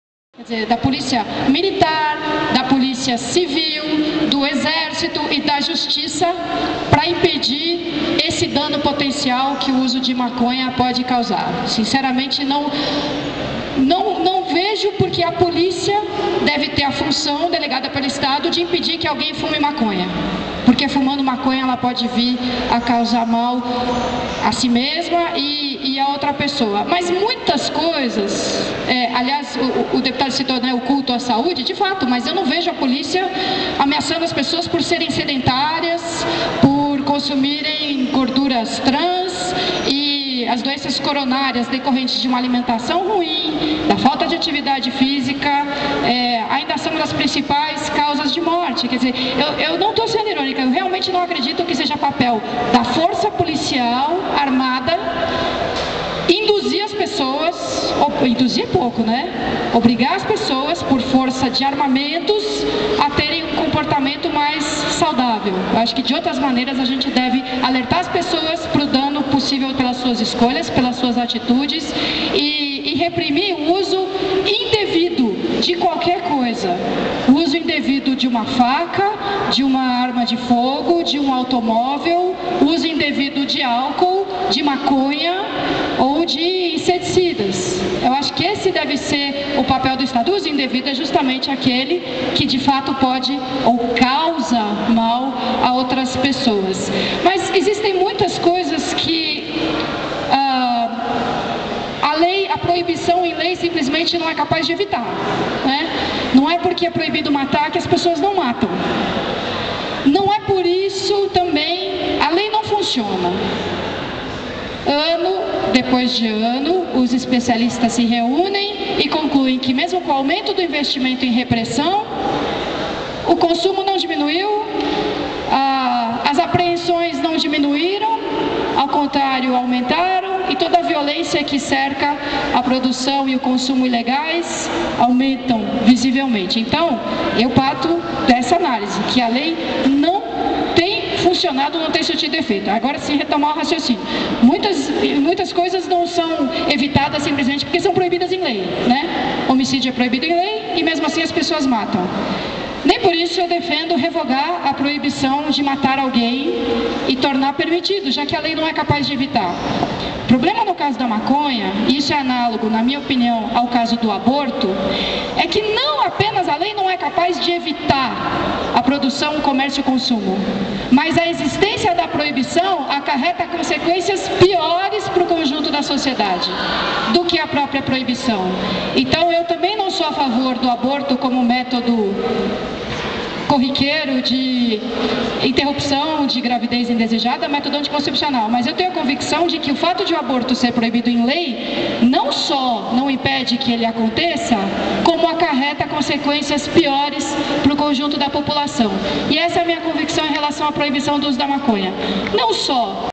Debate USP São Francisco Legalização da Maconha - Soninha Francine pt 2
Debate na São Francisco, dia 24/08/09.
Soninha Francine pt 2